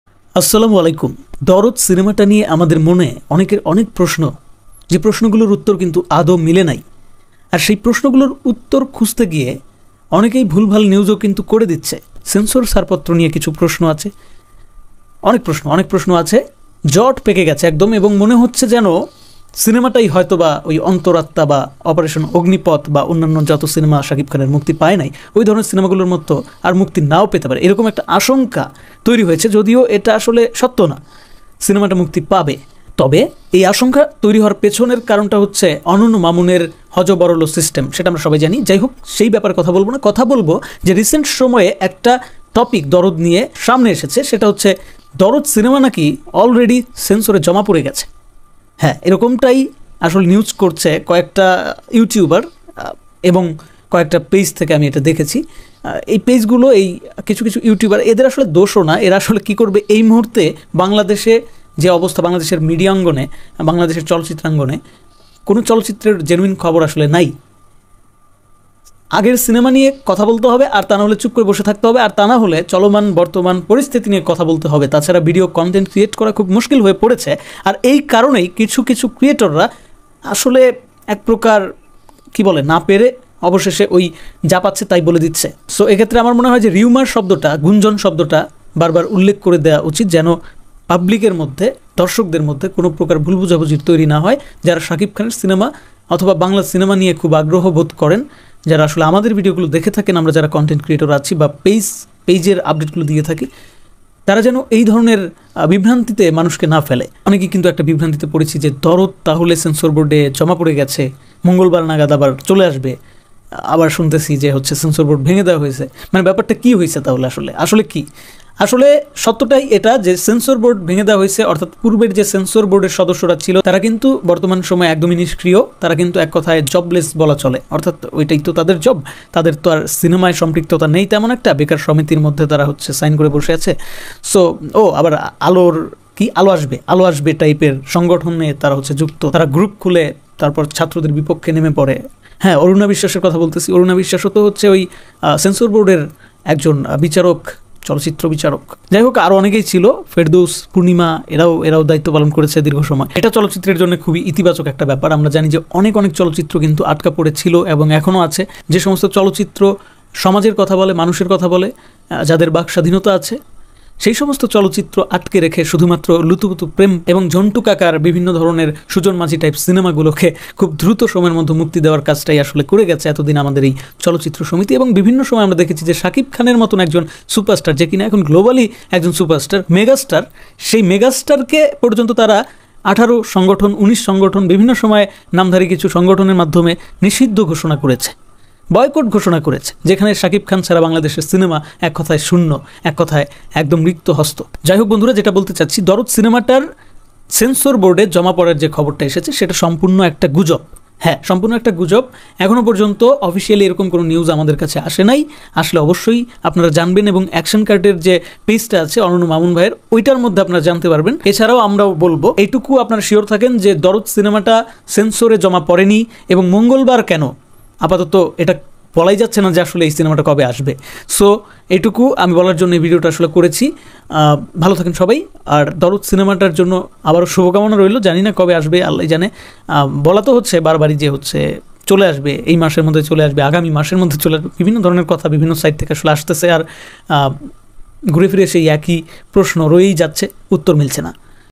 OMG! Shakib Khan এর দরদ(DARD) মুভি সেন্সরে জমা পরে গেছে❓
Anonno Mamunপরিচালিত শাকিব খানের প্রথম প্যান ইন্ডিয়ান সিনেমা Dorod ছবিটি সেন্সরবোর্ডে জমা হয়ে গেছে সম্প্রতি এমনটাই গুঞ্জন শোনা যাচ্ছে সামাজিক যোগাযোগ মাধ্যমে! ঘটনার সত্যতা কি? shakib khan এবং Sonal Chauhan এর দরদ ছবির নতুন আপডেট কি তা নিয়ে বিস্তারিত আলোচনা করলাম।
Shakib khaner chobi update
Pan indian movie dard prom
Dard Movie trailer shakib khan and sonal chouhan